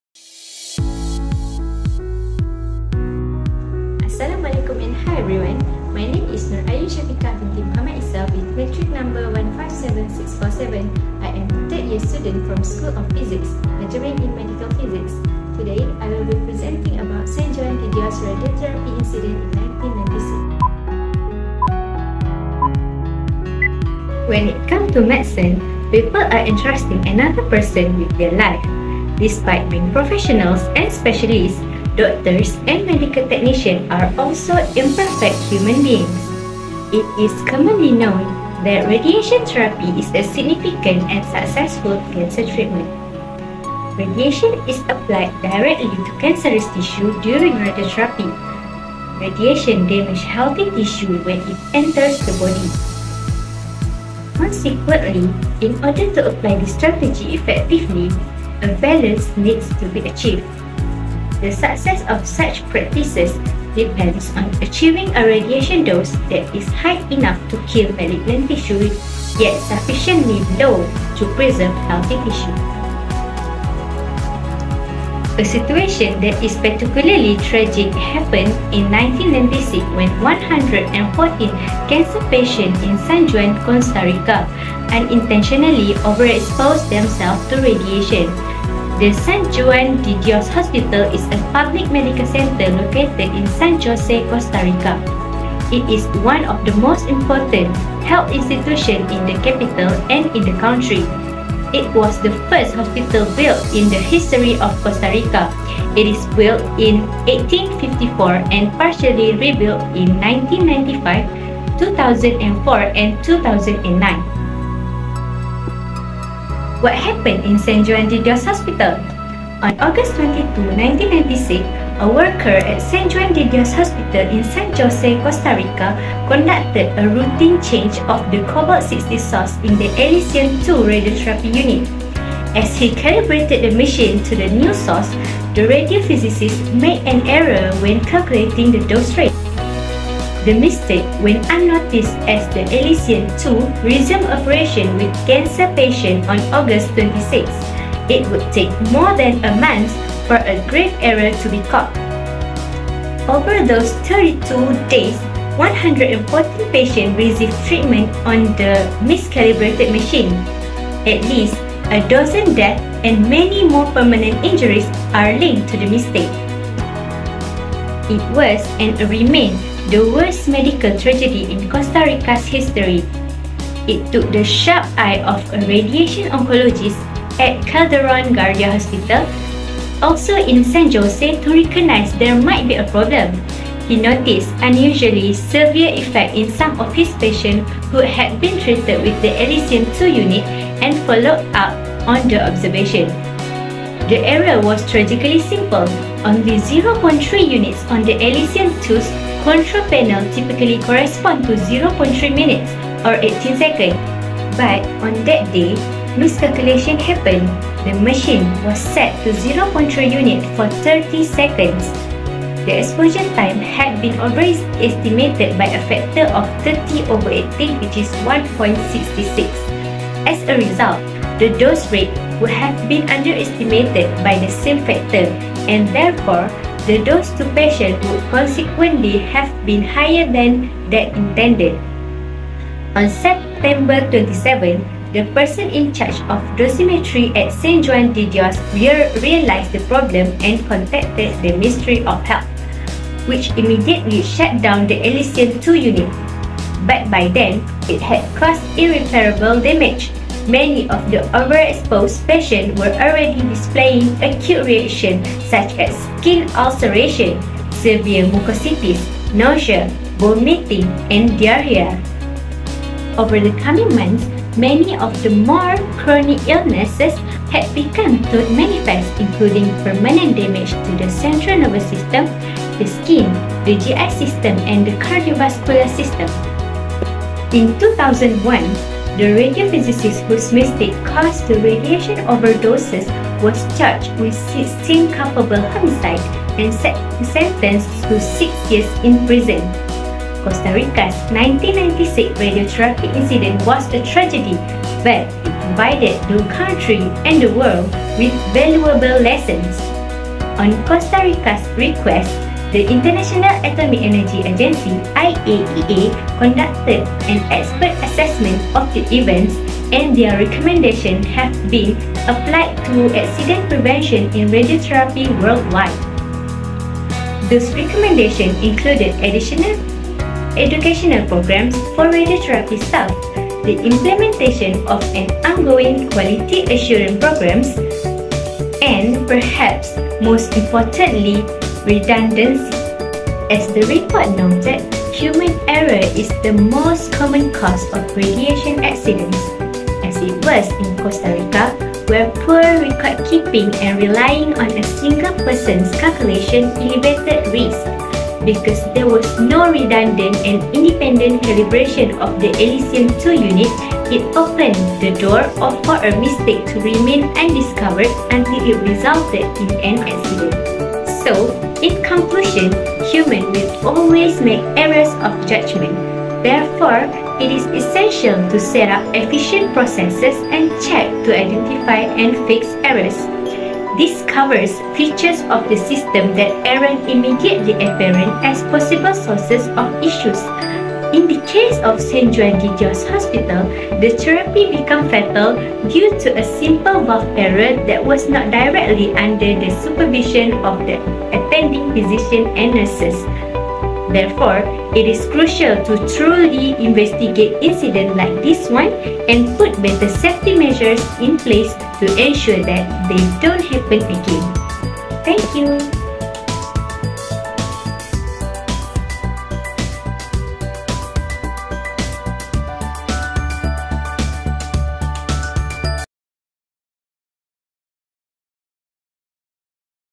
14) G14 - San Juan de Dios radiotherapy accident (1996)
ZMT 431/4 Radiation Biophysics (Semester 1, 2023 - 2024)
Presented by: GROUP 14
Members:
1) NUR AIYUN SYAFIQAH BINTI MOHD ISA (157647)
Together we share the knowledges